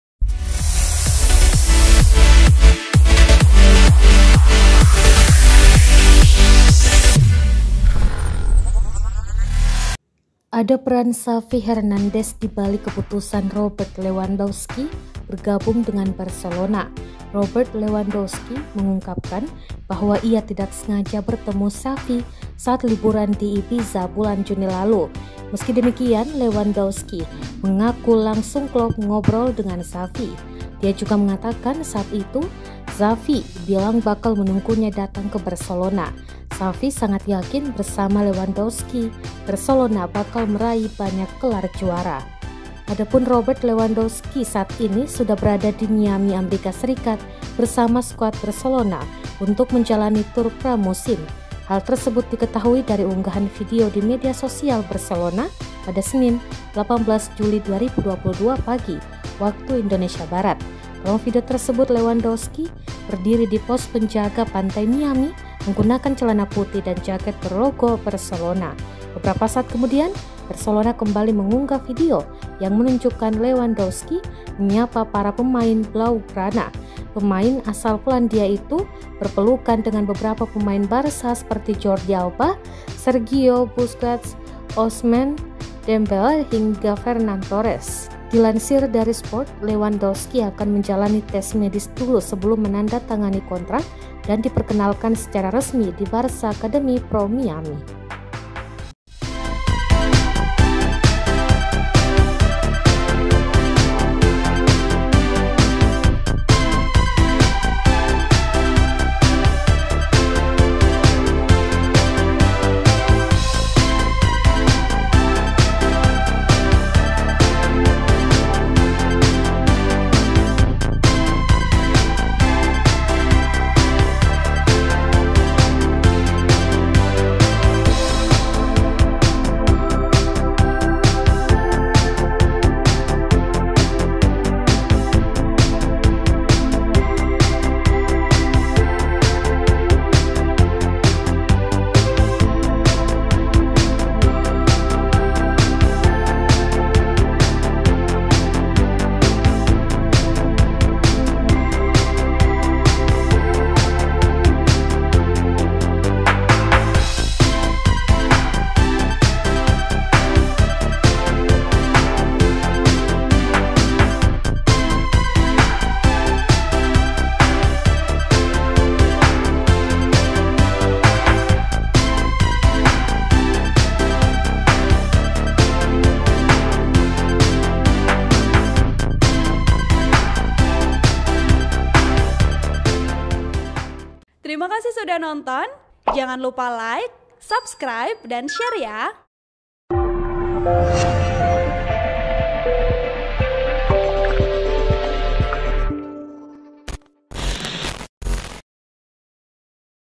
Pertemuan dengan Xavi Buat Lewandowski Melabuhkan Hati ke Barcelona
KOMPAS.com - Ada peran Xavi Hernandez di balik keputusan Robert Lewandowski bergabung dengan Barcelona.  Robert Lewandowski mengungkapkan bahwa ia tidak sengaja bertemu Xavi saat liburan di Ibiza bulan Juni lalu.

Meski demikain, Lewandowski mengaku langsung klop ngobrol dengan Xavi. Dia juga mengatakan, saat itu Xavi bilang bakal menunggunya datang ke Barcelona.  Xavi sangat yakin bersama Lewandowski, Barcelona bakal meraih banyak gelar juara.(TRIBUN-TIMUR.COM)

Sumber:  Kompas.com
Narasi: Nur Fajriani R
Editor: Rahman